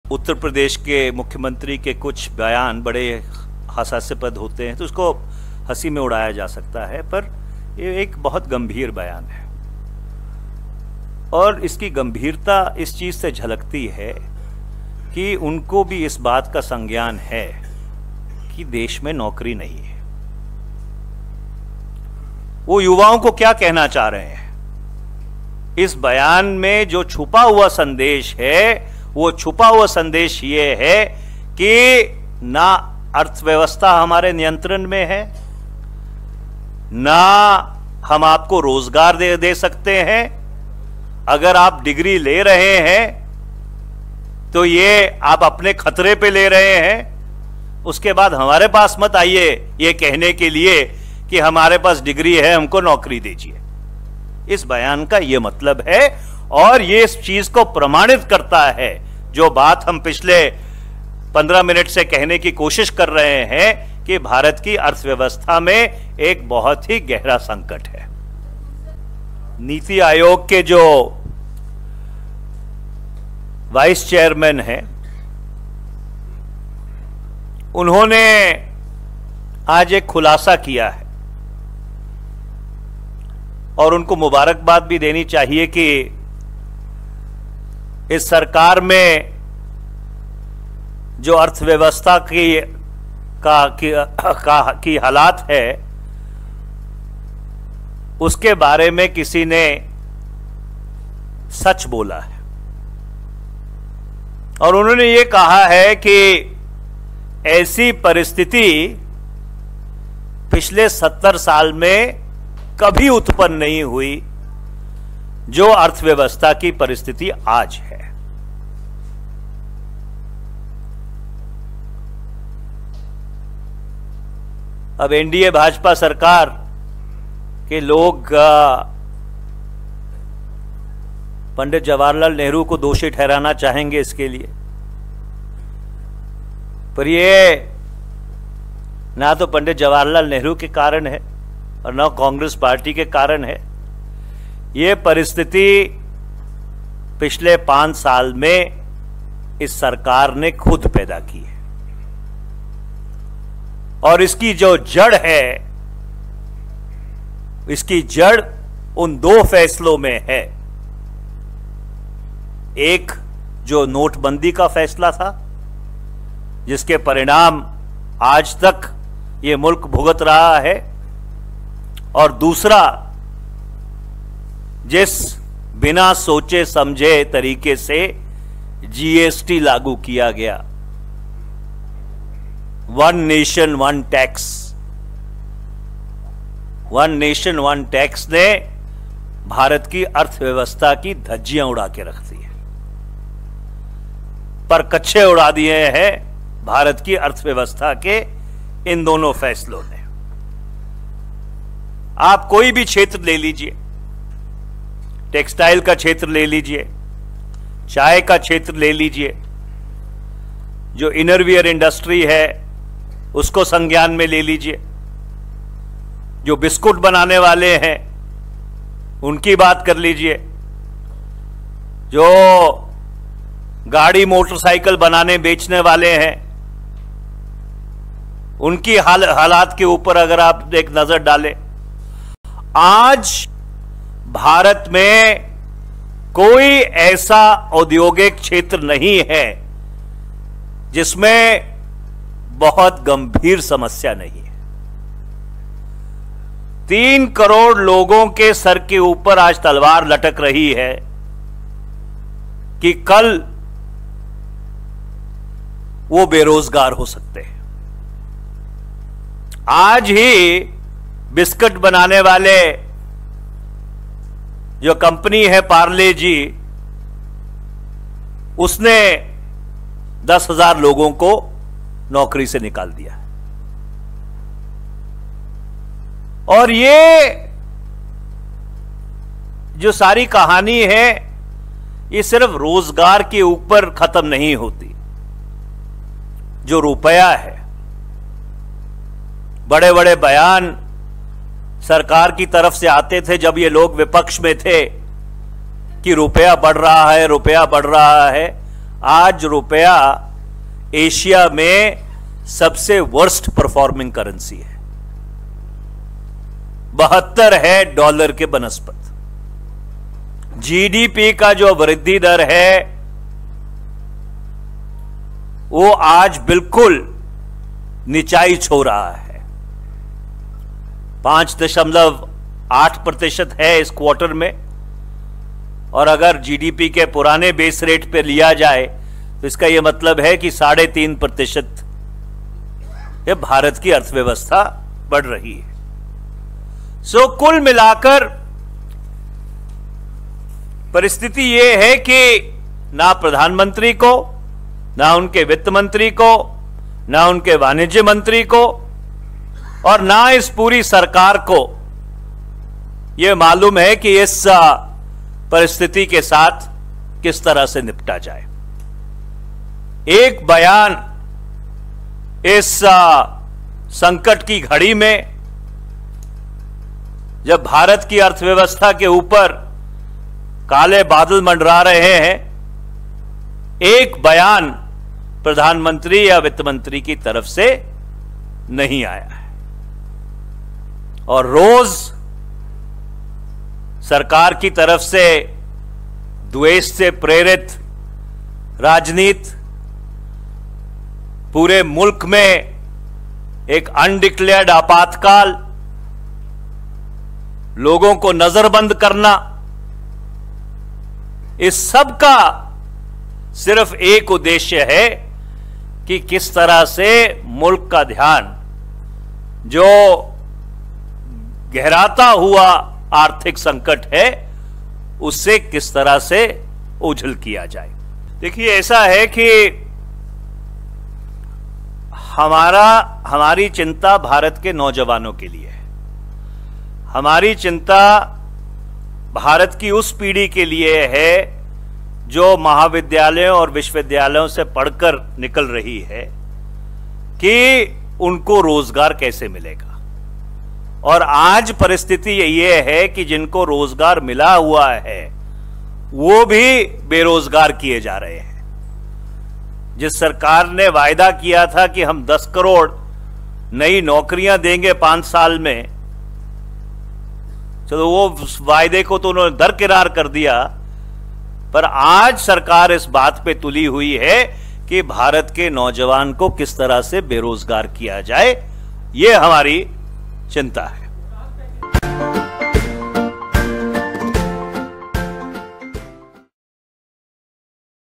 मंदी पर कांग्रेस के निशाने पर PM मोदी- गलत फैसलों ने उड़ा दी अर्थव्यवस्था की धज्जियां !
AICC Press Briefing By Manish Tiwari on Economic Crisis 2019

देश में मंदी का खतरा बढ़ता जा रहा है, ऑटो सेक्टर समेत कई सेक्टर से नौकरियां जाने का सिलसिला शुरू हो गया है. इस बीच नीति आयोग के वाइस चेयरमैन राजीव कुमार ने भी माना कि देश पिछले 70 सालों में ऐसी स्थिति का सामना नहीं किया था जैसा कि अब कर रहा है. राजीव कुमार ने कहा कि देश वित्तीय संकट से जूझ रहा है. कांग्रेस ने नीति आयोग के उपाध्यक्ष राजीव कुमार की तारीफ करते हुए कहा कि देश की अर्थव्यवस्था पर सच बोलने के लिए राजीव कुमार बधाई के पात्र हैं.उन्होंने स्वीकार किया है कि अर्थव्यवस्था में ऐसी परिस्थिति 70 साल में कभी उत्पन्न नहीं है जो आज है.